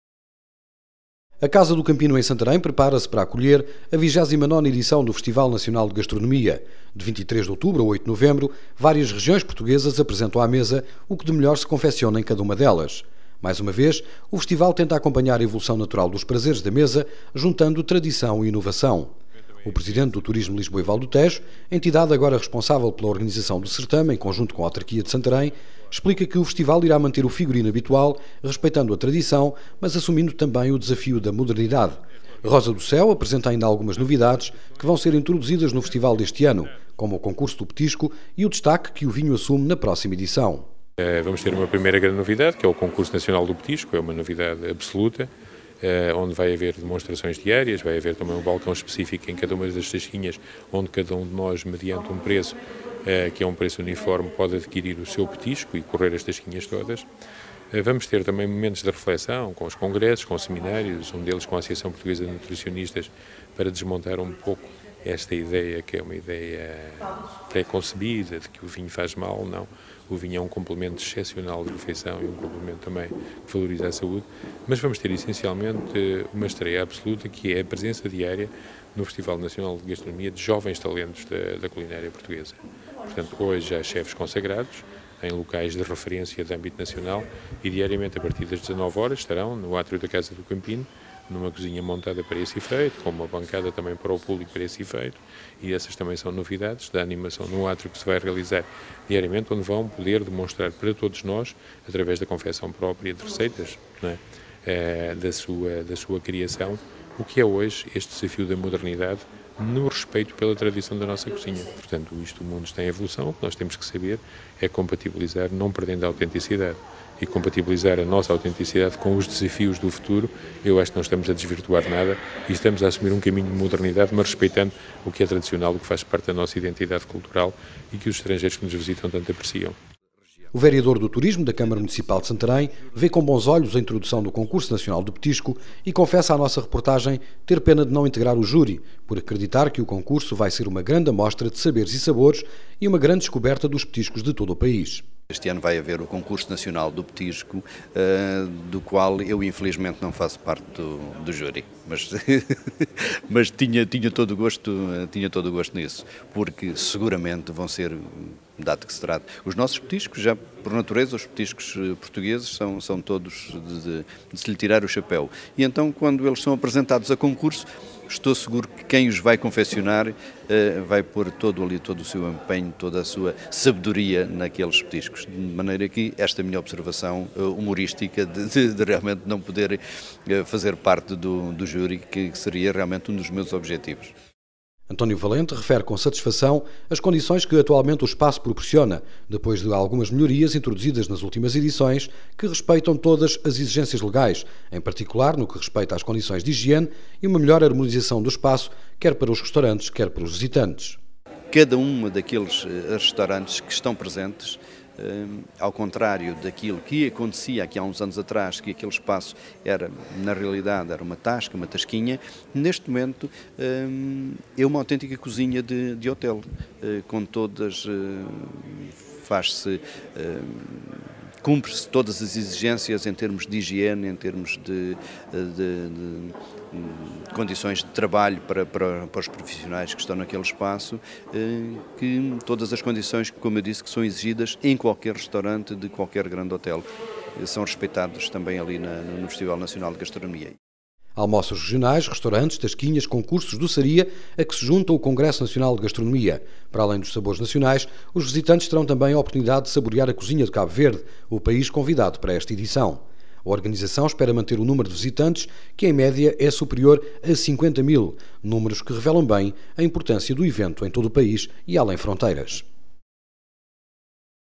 Santarém acolhe mais um festival nacional de gastronomia
Entre 28 de Out. e 8 de Nov. são esperados mais de 50 mil visitantes